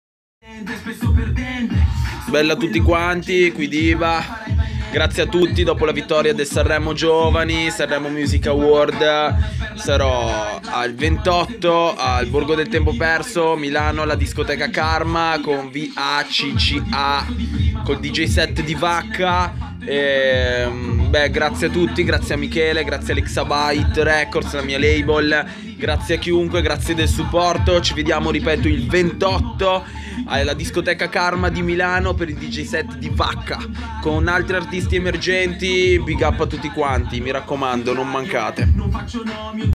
DIBA Vincitore Di Sanremo Giovani al Karma Sabato 28 Maggio
DIBA VINCITORE SANREMO GIOVANI SABATO 28 MAGGIO KARMA - EX BORGO DEL TEMPO PERSO - SPECIAL GUEST VACCA DJ SET - BIG DJ LUCA MARENZI SALA TECHNO SERATA IN DA CLUB !